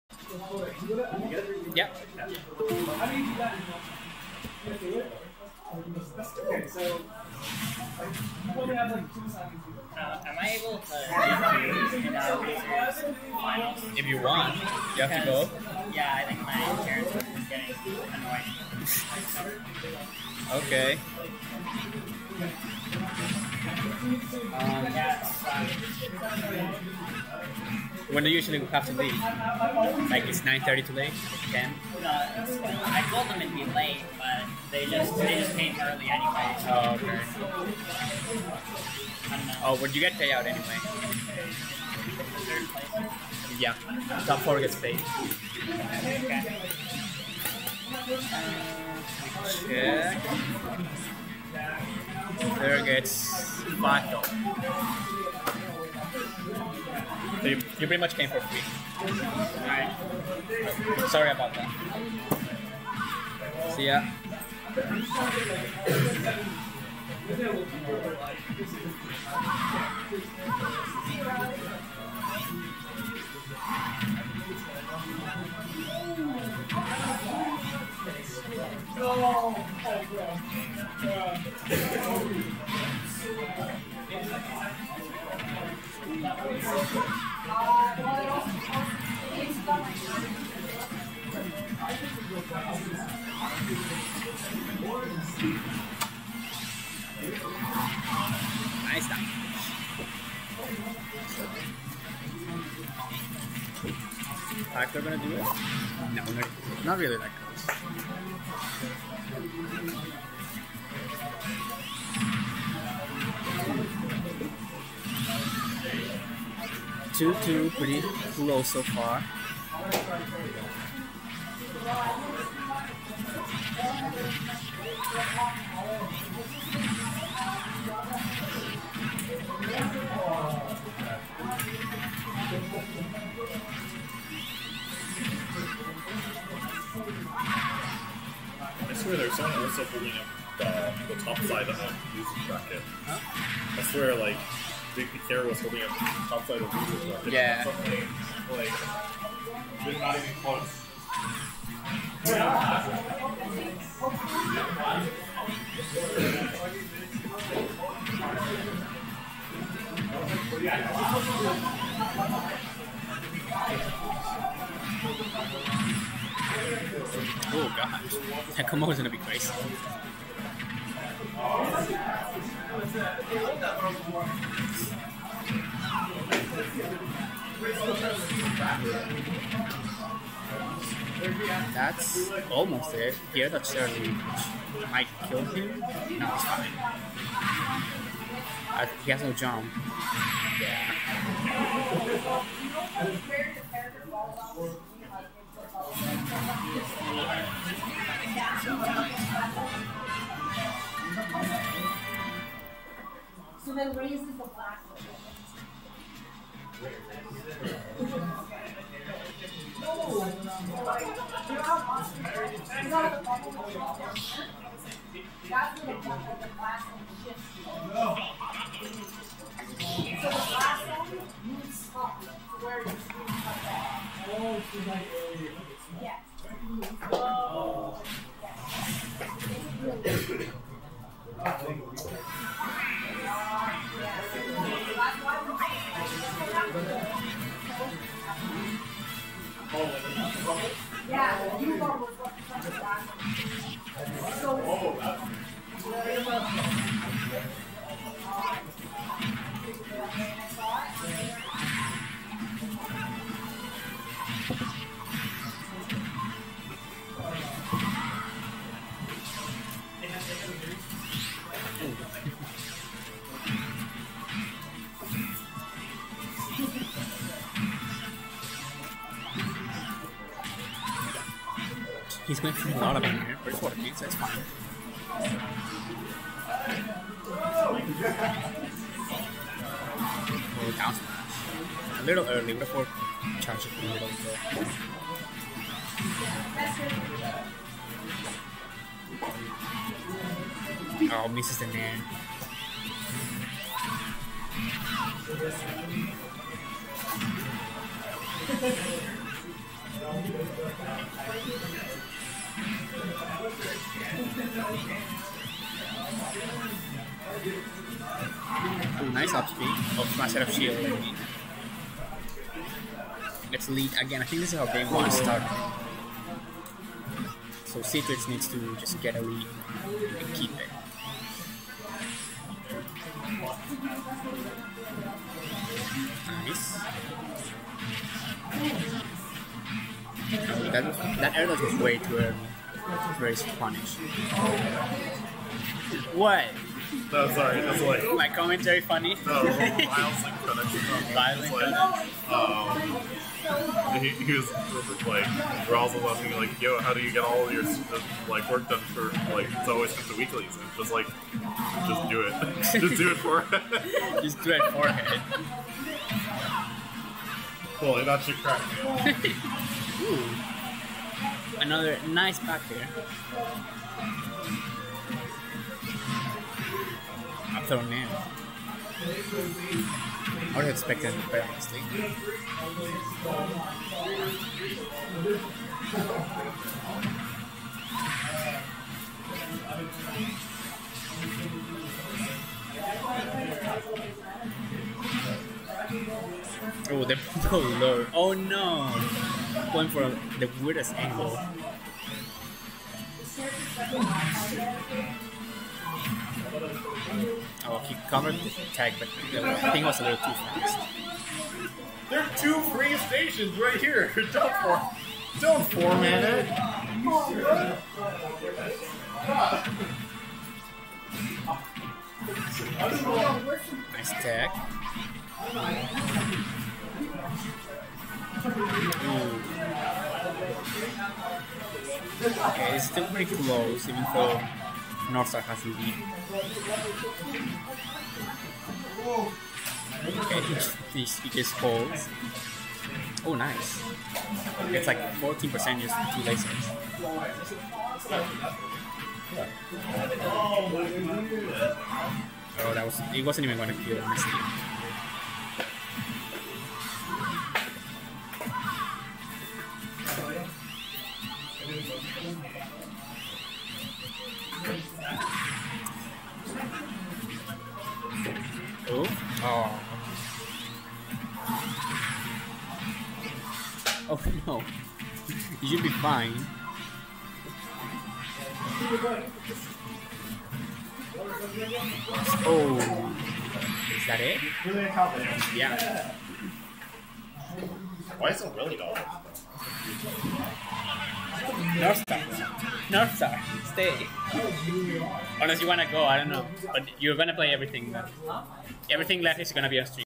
Mac is Back #2 - NorthStar vs Ceatrix - Losers Quarters
McMaster weeklies happen every wednesday, on McMaster Campus. for more details!